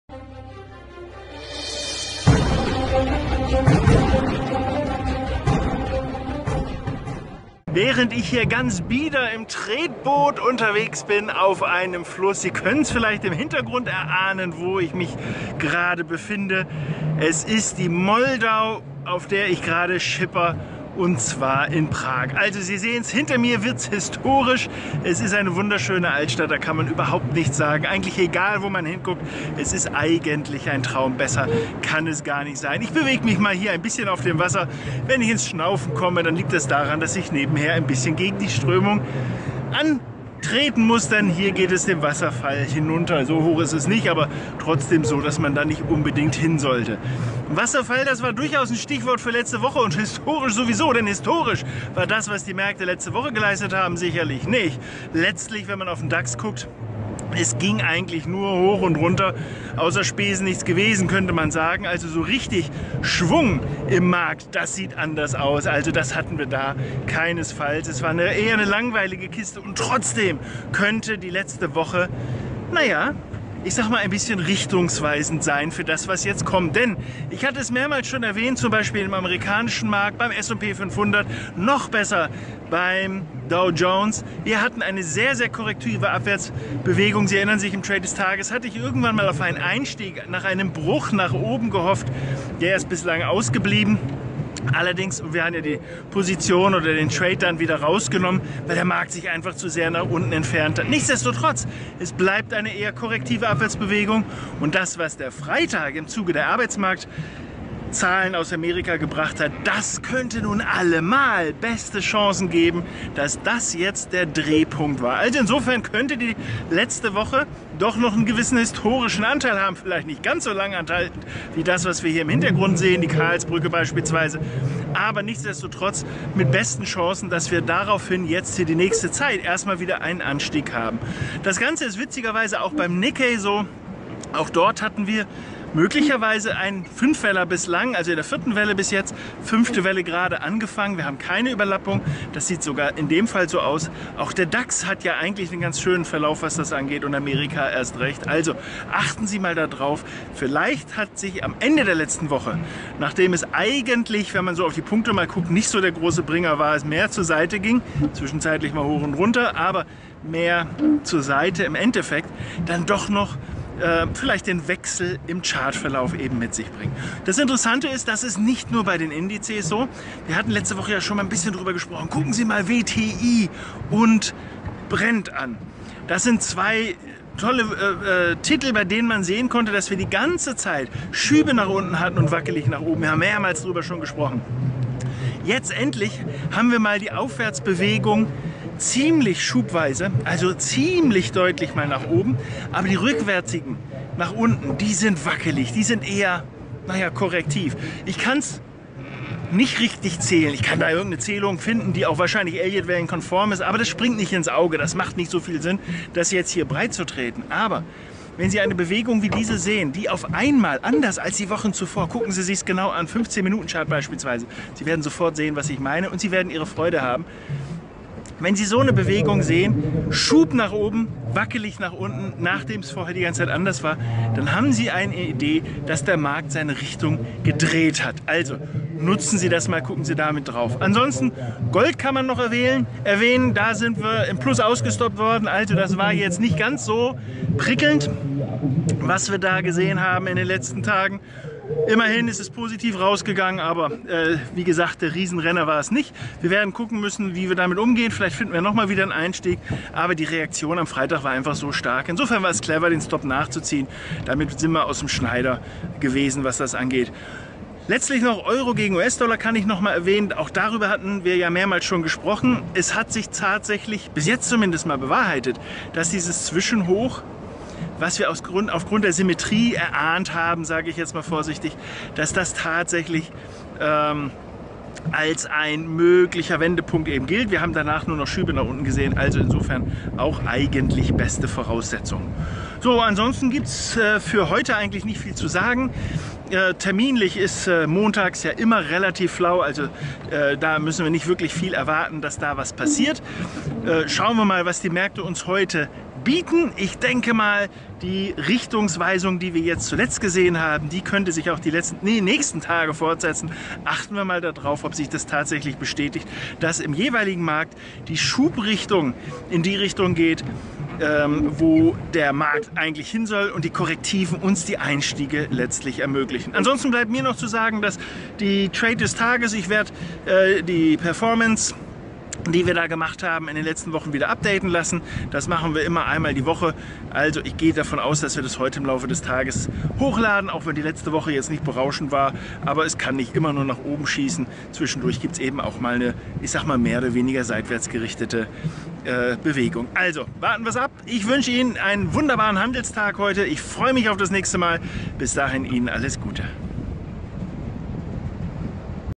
Rüdiger Born: Dax, Öl, Euro
Im Dax ging es letzte Woche nur rauf und runter. Schwung sieht anders aus. Trotzdem könnte die letzte Woche richtungsweisend sein für das, was jetzt kommt. Bei WTI + Brent gucken Sie bitte mal in den 15 Minuten-Chart - Schub nach oben, wackelig nach unten. Sehr interessant! Auch zum Euro mehr im Video...